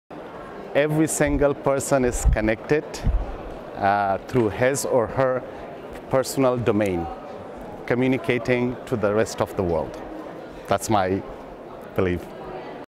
Global IGF 2017: Zmarialai Wafa on the Internet’s Future in a Nutshell
Zmarialai Wafa, ICT specialist with Internet Society Afghanistan, shares their answer to the question, “In just a word or a brief phrase, in 5 seconds or less, describe the Internet’s future?” Recorded at the Global Internet Governance Forum 2017 in Geneva, Switzerland, by the Imagining the Internet Center, an initiative of Elon University, North Carolina, USA.